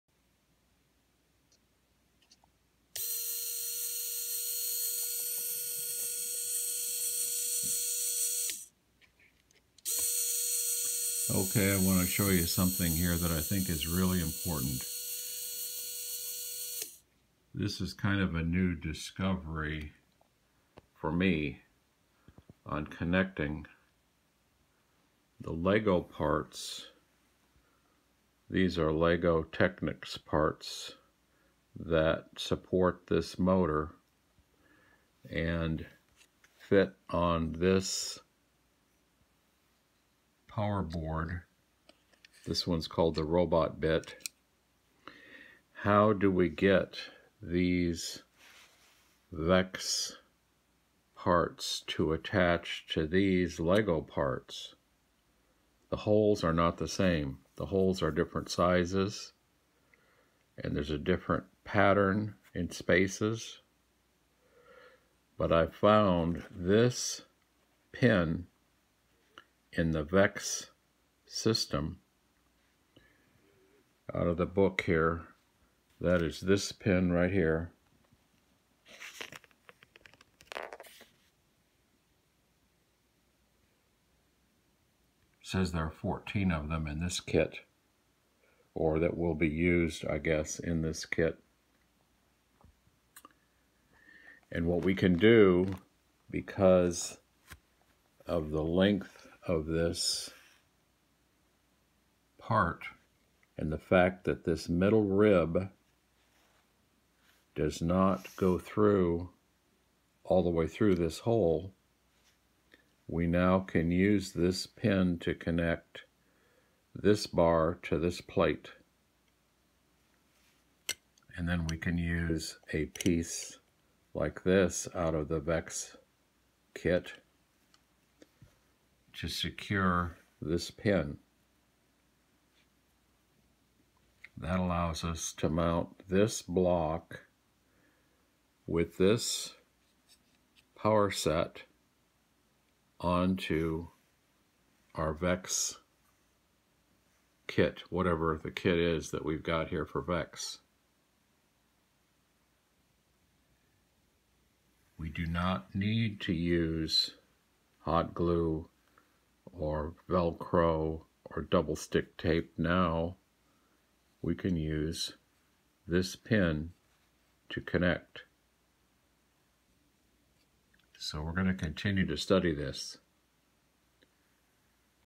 Connecting VEX parts to Lego Technic parts with VEX pins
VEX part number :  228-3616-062
Capped Connectors also work. Capped 0X3 works best